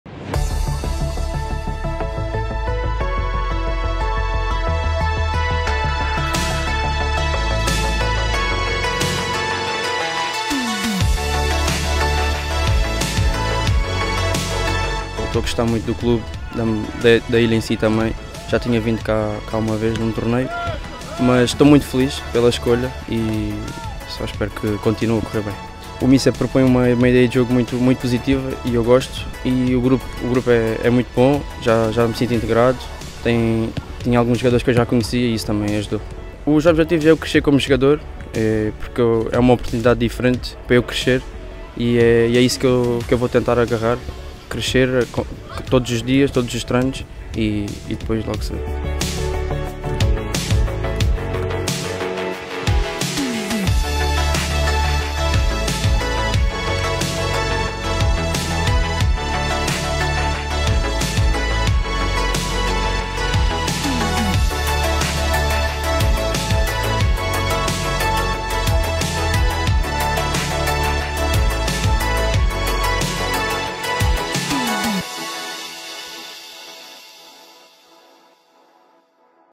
🎙​​💬 Rafael Brito: “𝙀𝙨𝙩𝙤𝙪 𝙖 𝙜𝙤𝙨𝙩𝙖𝙧 𝙢𝙪𝙞𝙩𝙤 𝙙𝙤 𝙘𝙡𝙪𝙗𝙚 (...) 𝙚𝙨𝙩𝙤𝙪 𝙢𝙪𝙞𝙩𝙤 𝙛𝙚𝙡𝙞𝙯 𝙥𝙚𝙡𝙖 𝙚𝙨𝙘𝙤𝙡𝙝𝙖”
2ª SEMANA DE PRÉ-ÉPOCA ⚽🔛
Ínicio de mais uma semana de trabalho no Complexo Desportivo do Marítimo. 🔋⚡💪